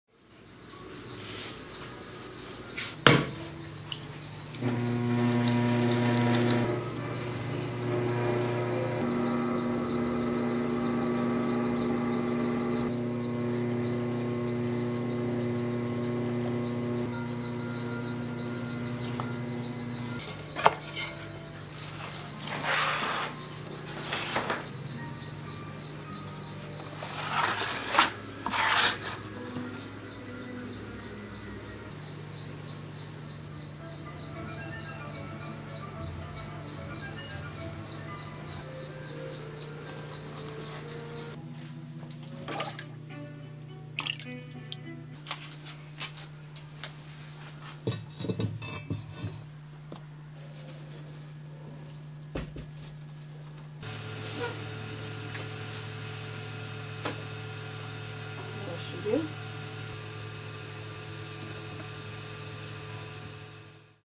Down Under Pottery
A short narration-free documentary on the creation of bowls at Down Under Pottery in Lincoln, Nebraska.